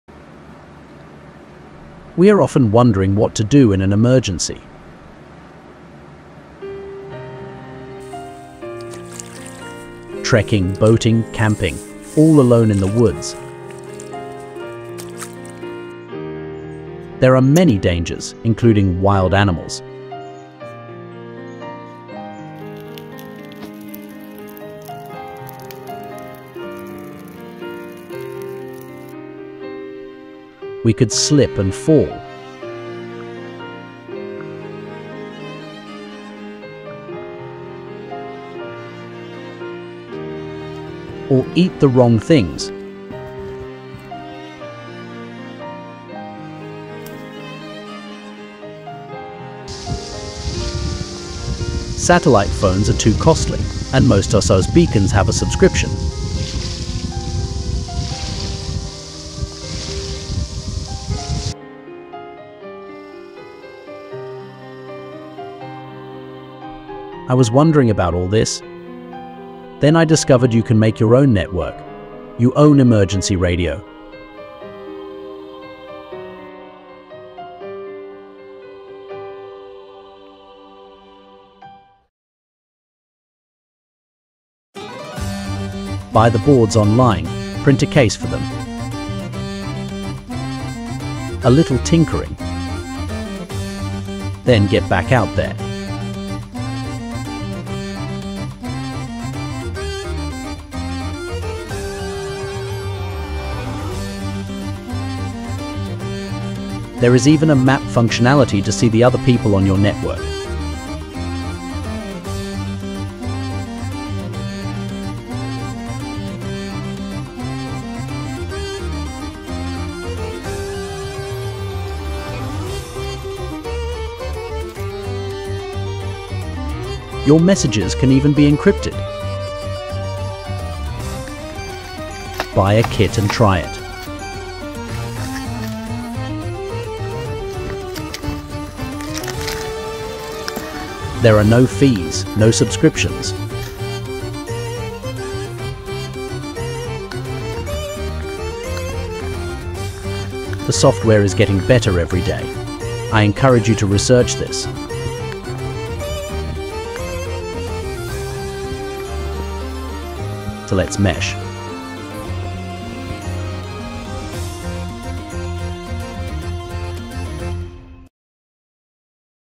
Comms for Hiking, Boating, Trekking? MESHTASTIC
in places when cellphones arent working you could instead use your own network. buy a couple of nodes and join the mesh. perfect for emergency network. support encryption!
MESHTASTIC